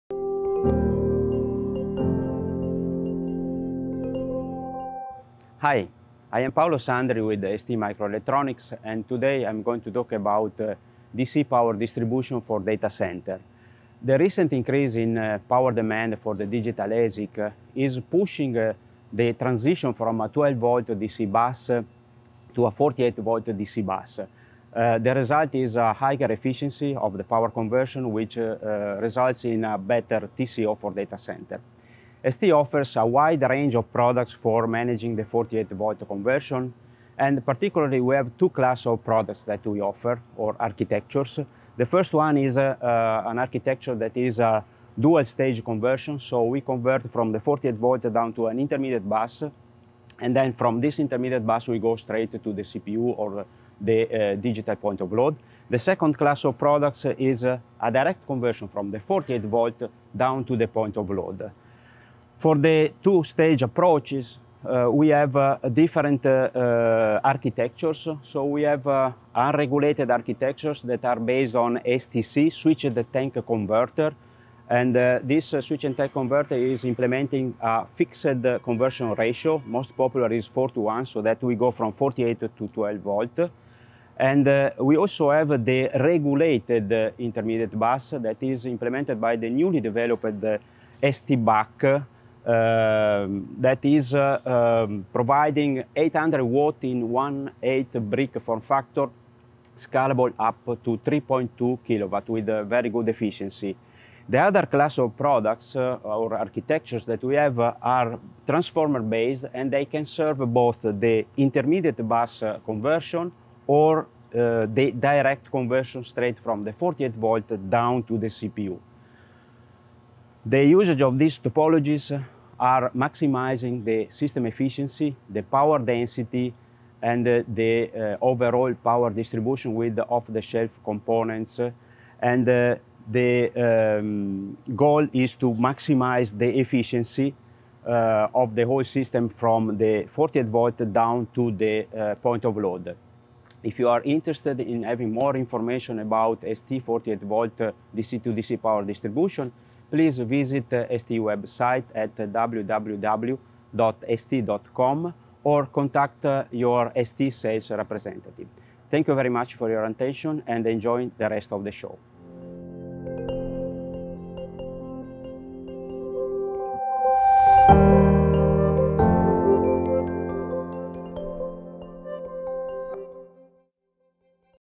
DevCon 2020 Presentation: Efficient Power Delivery for Modern Data Center
Complete power distribution for servers from AC input to DC PoL maximizing overall efficiency and power density. The AC-DC stage delivers 500 W at 48 V, followed by ST proprietary architecture (Stacked Buck) to convert 48 V to regulated 12 V, and a multiphase digital architecture to power digital ASIC.